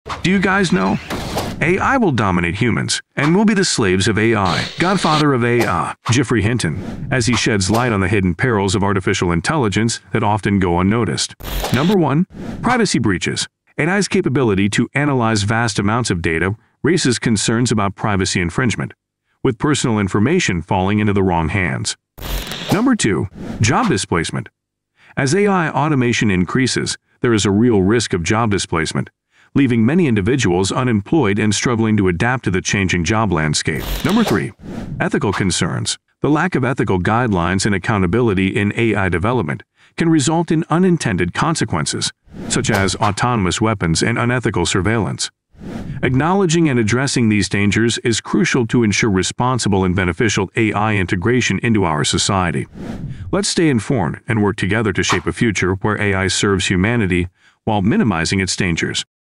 AI Attacking Humans: Geoffrey Hinton, Godfather of AI already Warns About It !
In this captivating YouTube Shorts video, we delve into the alarming topic of AI attacking humans and the cautionary words of Geoffrey Hinton, the renowned Godfather of AI. Join us as we explore the potential risks and ethical implications surrounding artificial intelligence's evolution. Discover Hinton's insightful predictions and expert advice on mitigating these dangers. Don't miss this thought-provoking video on a crucial subject shaping our future!